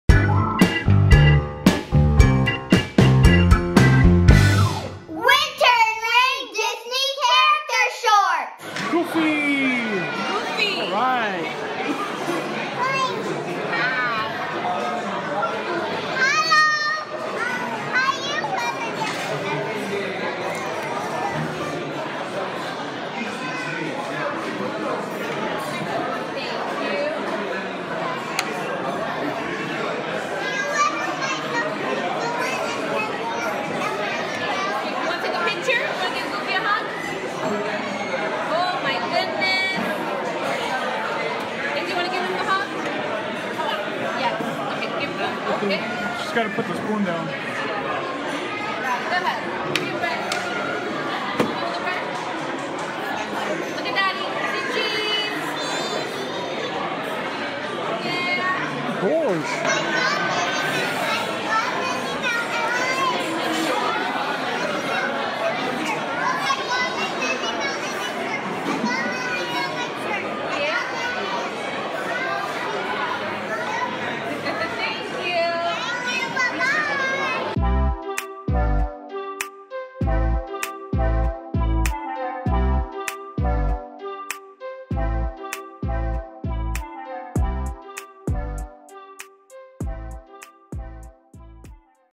Goofy Meet and Greet In Animal Kingdom | Disney World 2016
In this video: We meet Goofy in Animal Kingdom as part of the Tusker House character dine. I don't know what's up with the lady in the background looking like she wants to talk to Goofy, and PhotoPass didn't take pics so it's just video. I remember it being quite hot that day, so it was nice to cool off and meet some characters.

JOIN US ON INSTAGRAM & TWITTER: @sillysweetgirls